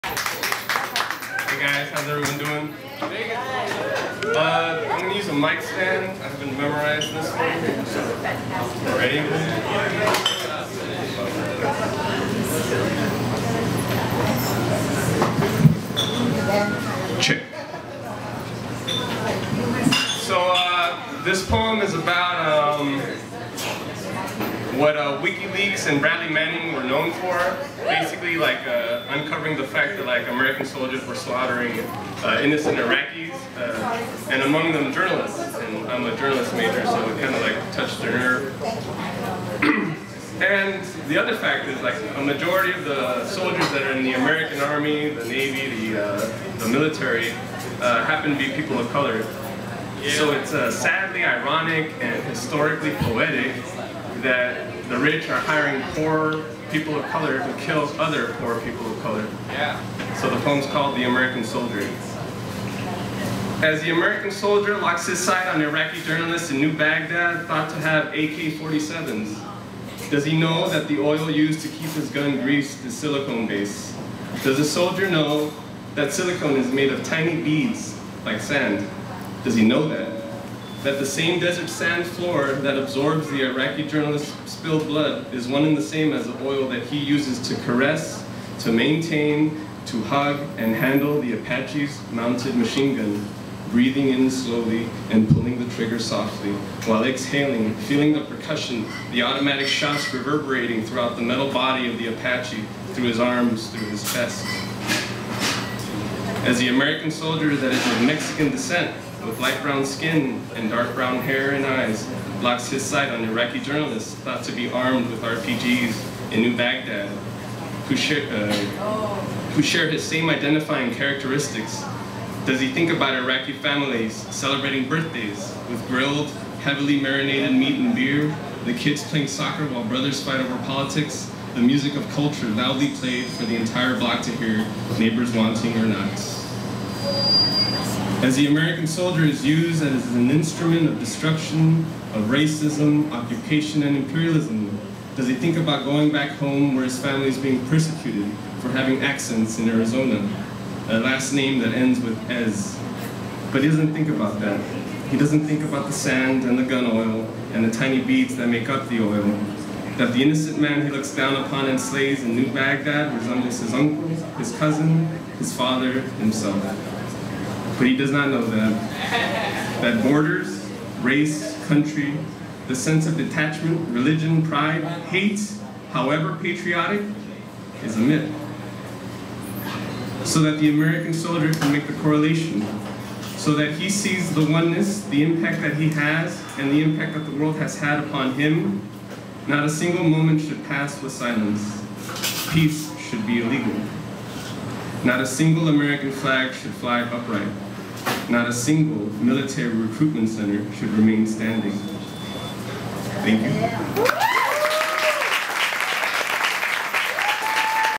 Luis Rivas - The American Soldier
Luis Rivas reciting the poem "The American Soldier" at the Whittier Peace & Justice Open Mic at the Fenix 5-4 (best place for beers) on Sunday night July 29, 2012.